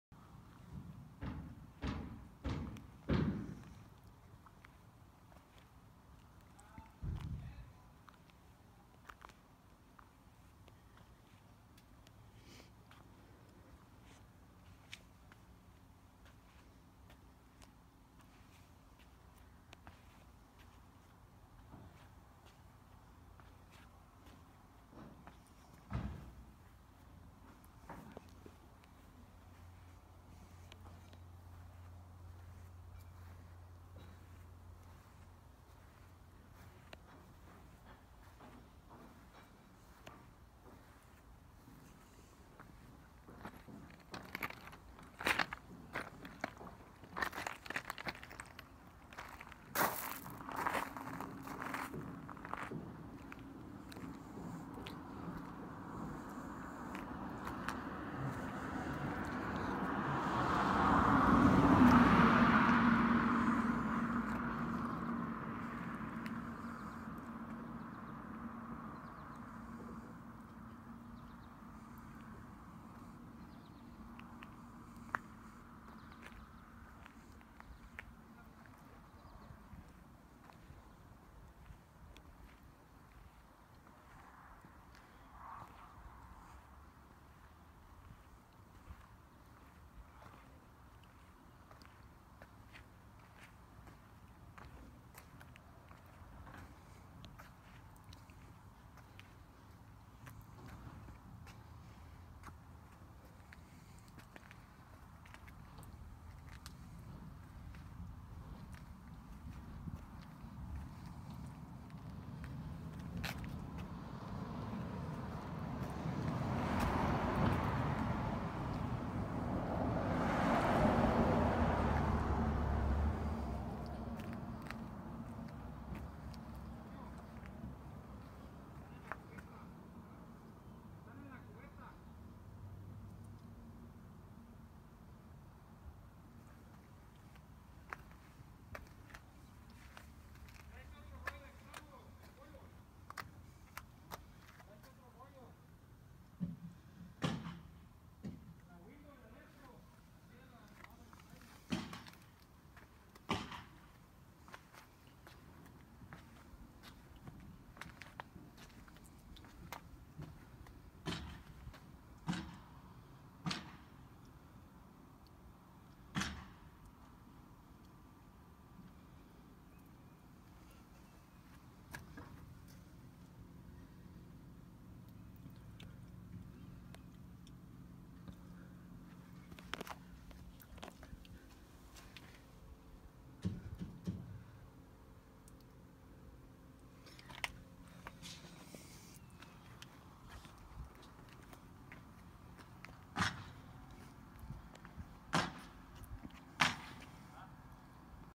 Lifted Ram 2500 HEMI ON 37s Gear Alloy 22x12
2016 Ram 2500
Zone 5.5" lift
37x13.50x22 Nitto Trail Grappler